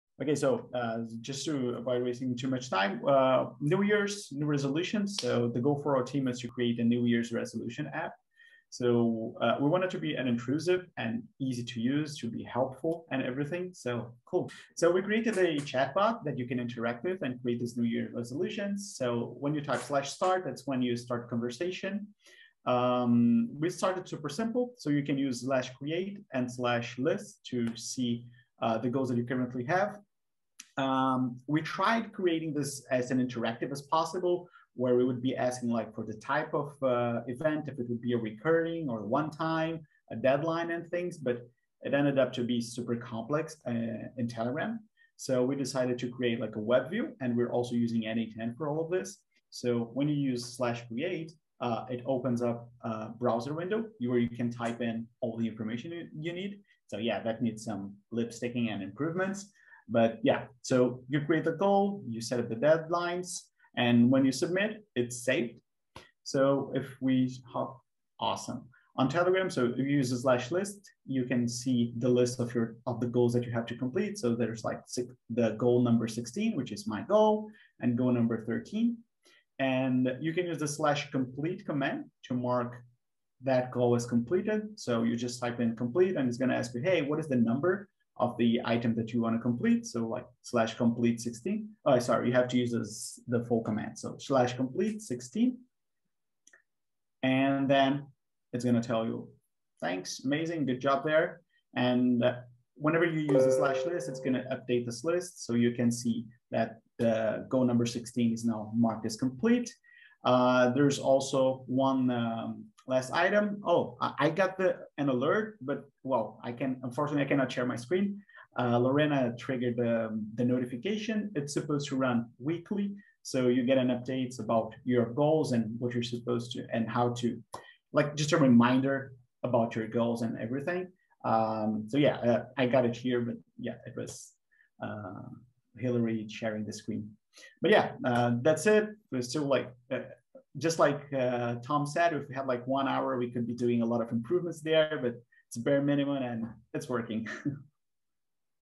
Create a Telegram bot to track resolutions
Hilary, Lorena, and Omar created a Telegram bot to track your resolutions.

Hackmation is an internal company hackathon where we use n8n to automate our tasks or build low-code products.